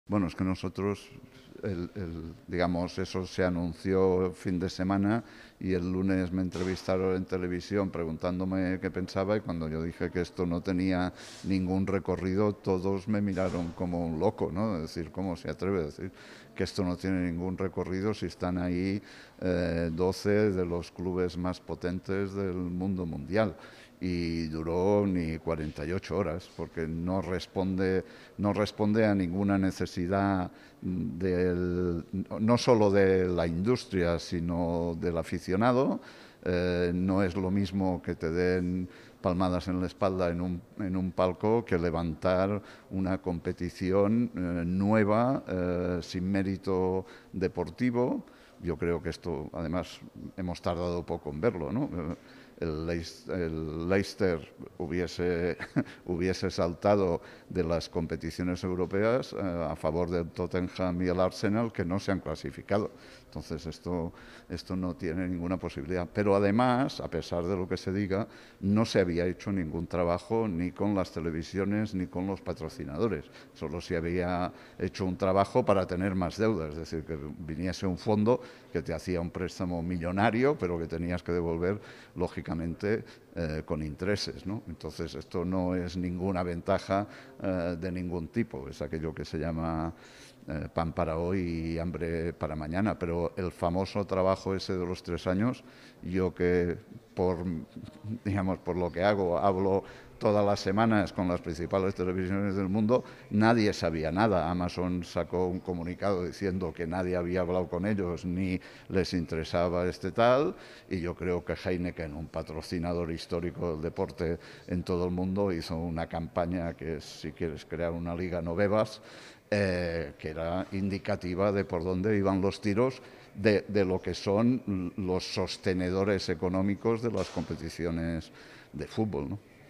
Roures 'atiza' a la Superliga: "No es igual a que te den palmaditas en un palco..." | Diario AS
Jaume Roures acudió al PRO Foro Industria y Deporte, organizado por 2PlayBook y AS, y habló del proyecto de la Superliga europea.